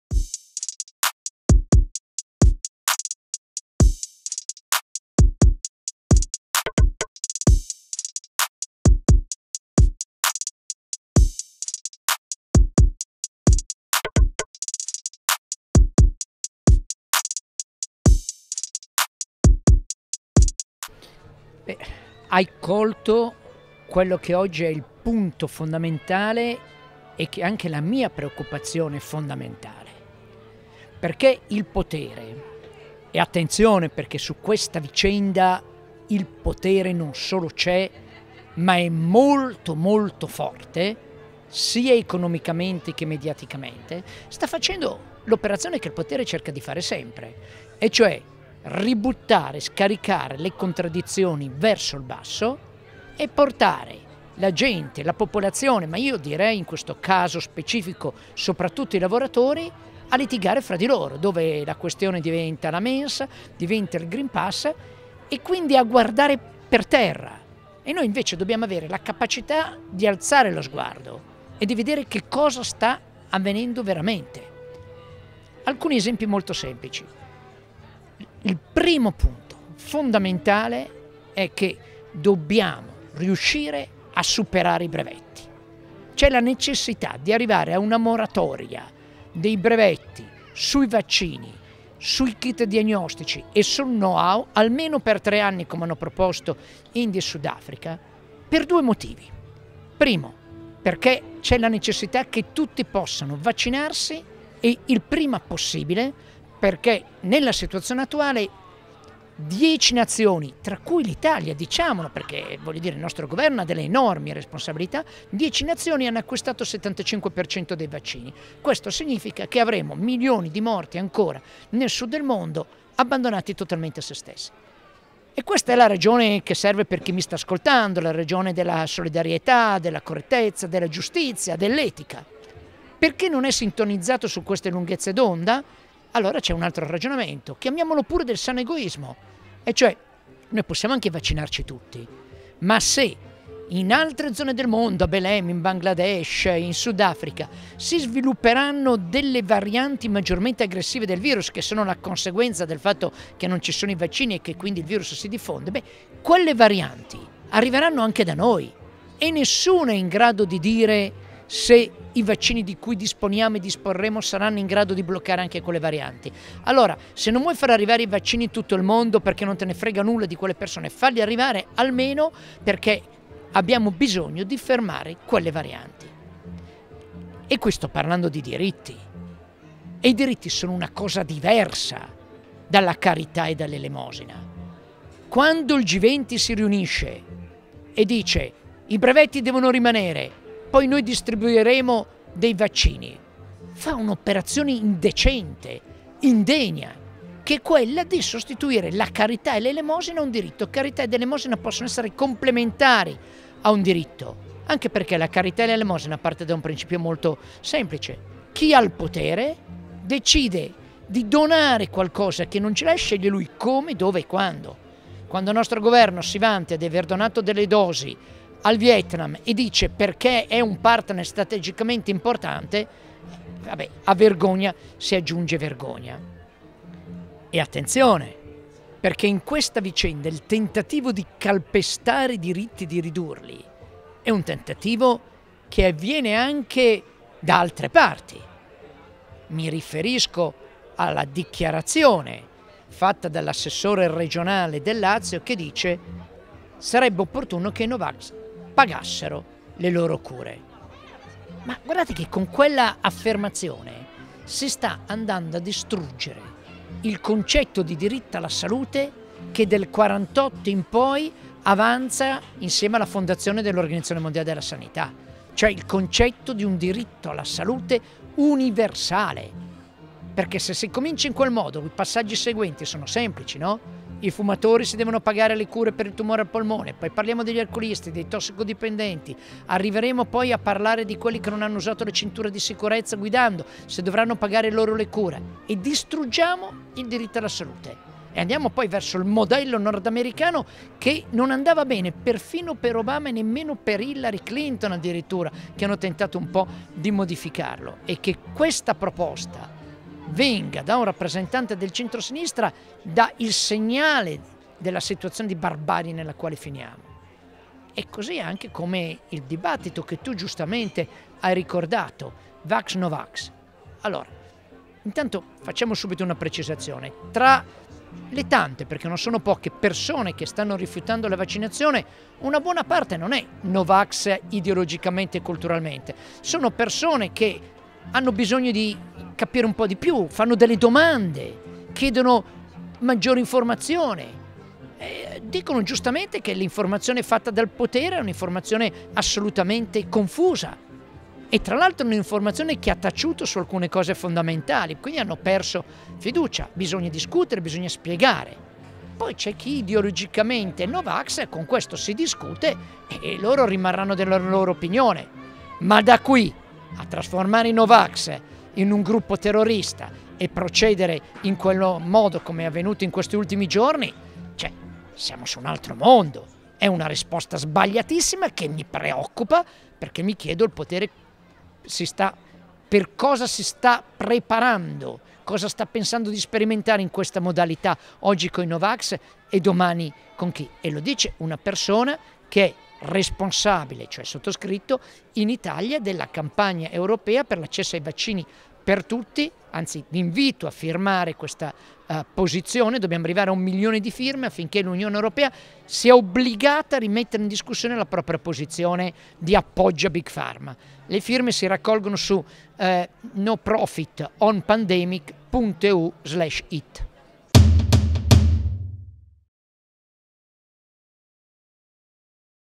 OEF *21 | Intervista a Vittorio Agnoletto | I movimenti sociali e la crisi sanitaria
Oltre Economia Festival | 10-12 settembre 2021 | Parco Santa Chiara, Trento